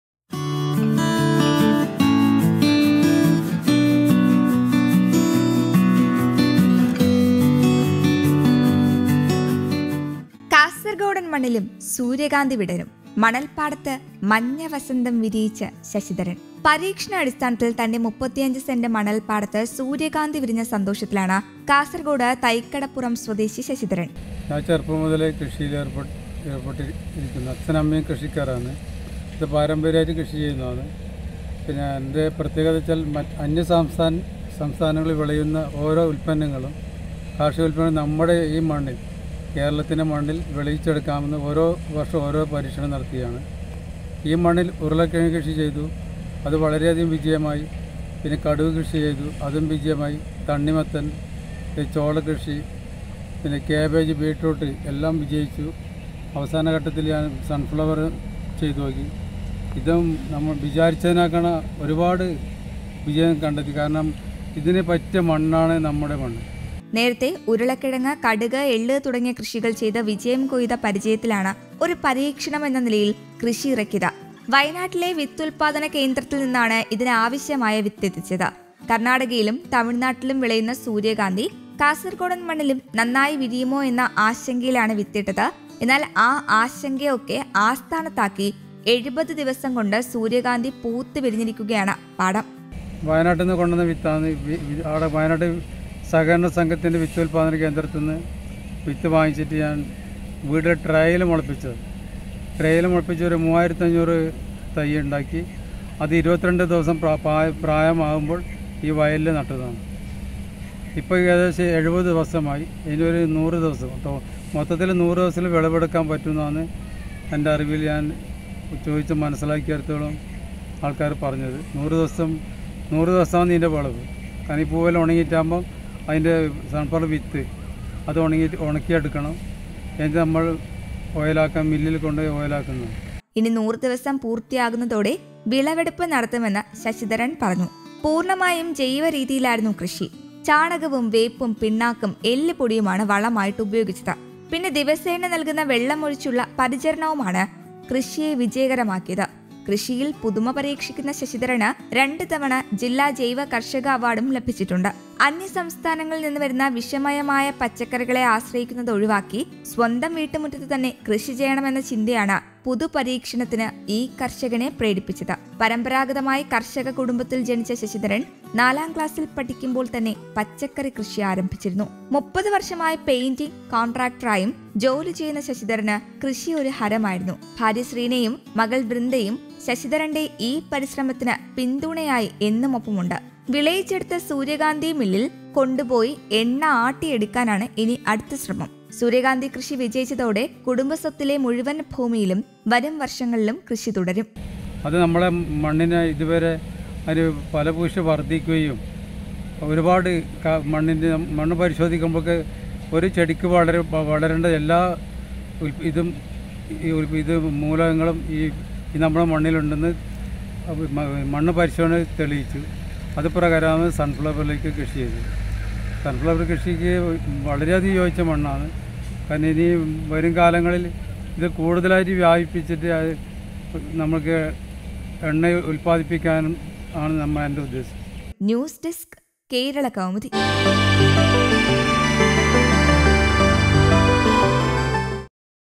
കാസർകോടൻ മണ്ണിലും സൂര്യകാന്തി വിടരും,മണൽ പാടത്ത് മഞ്ഞ വസന്തം വിരിയിച്ച് ശശിധരൻ| Sunflower | Kasargod
കാസർകോടൻ മണ്ണിലും സൂര്യകാന്തി വിടരും, മണൽ പാടത്ത് മഞ്ഞ വസന്തം വിരിയിച്ച് ശശിധരൻ